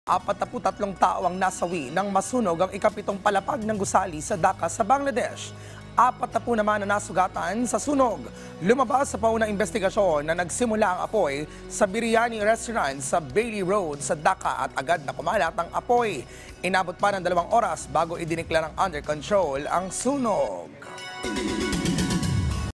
43 patay sa nasunog na gusali sa Bangladesh
43 patay sa nasunog na gusali sa Bangladesh